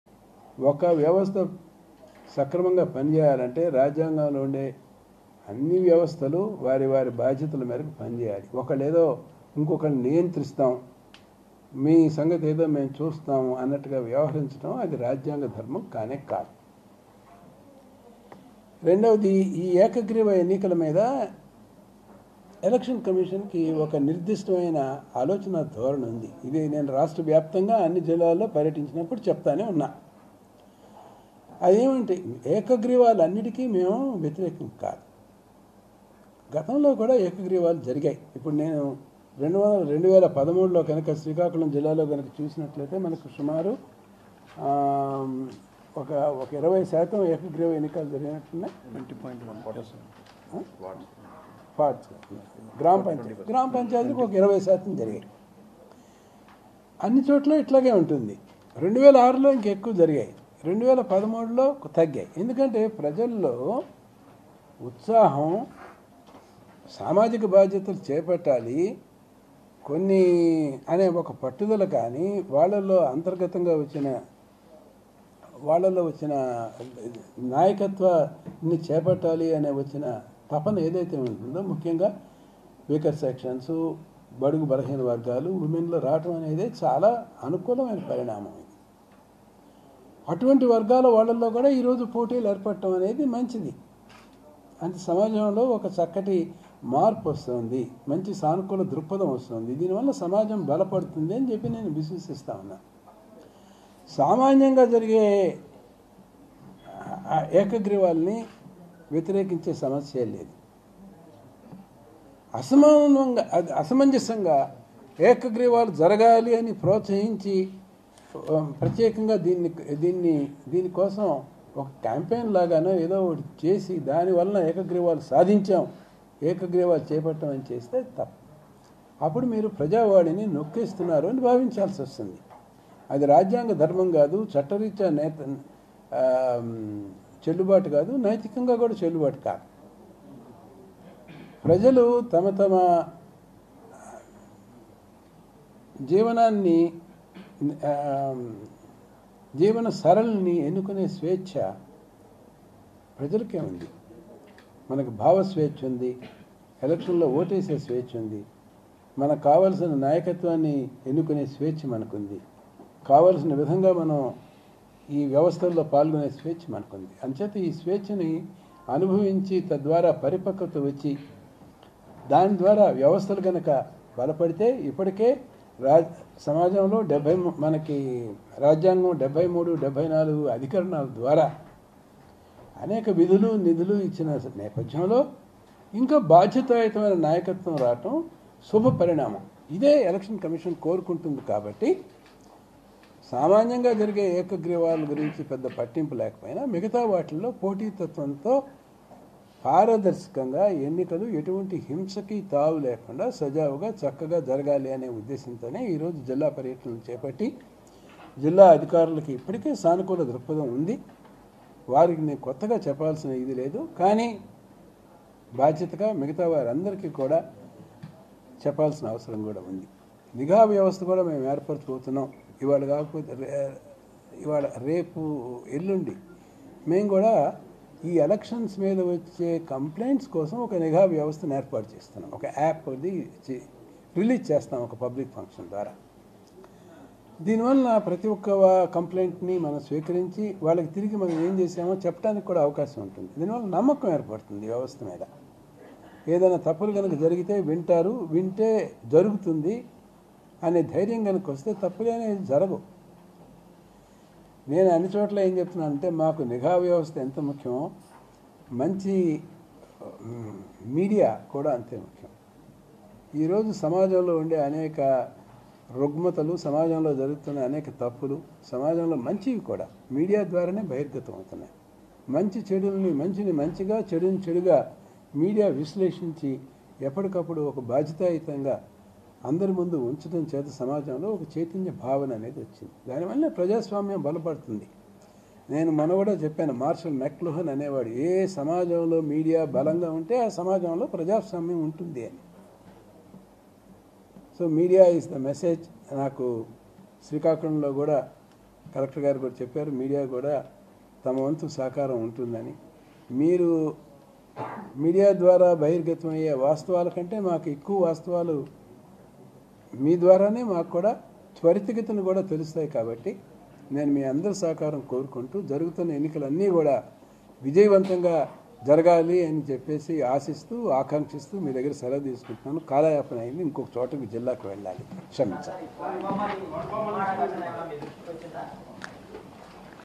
AP SEC Nimmagadda Ramesh Kumar on Unanimous Election | YSRCP Vs TDP | AP CM Jagan | ABN News
LIVE: AP SEC Nimmagadda Ramesh Kumar Press Meet Over AP Local Body Elections || ABN LIVE